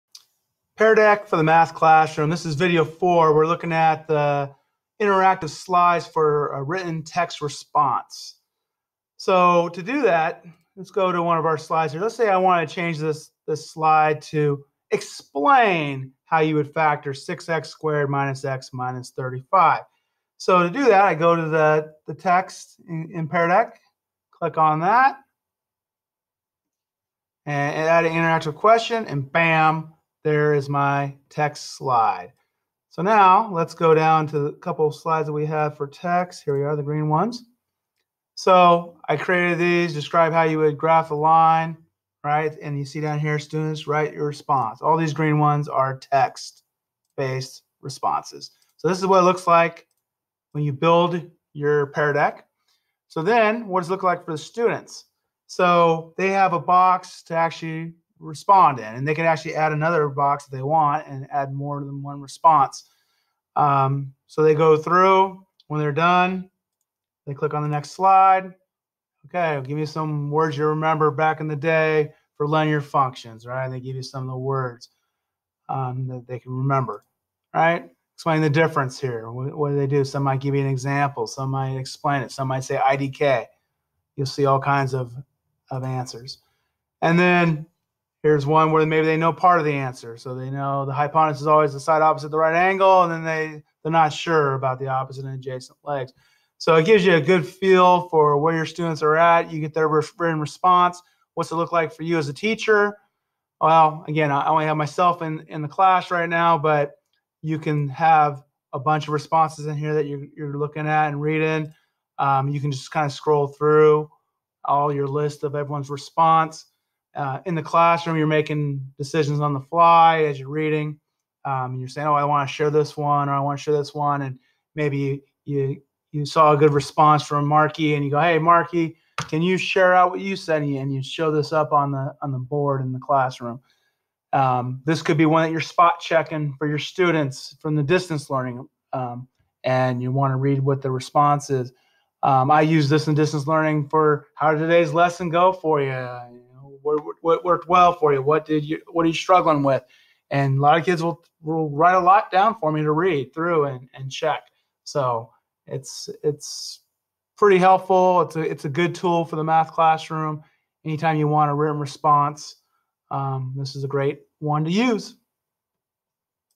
Pear Deck For Math Classroom Video 4 (Interactive Written Response Slides)
Pear Deck for Math Classroom Video 4. This video demonstrates how to create written response interactive slides for Math classroom.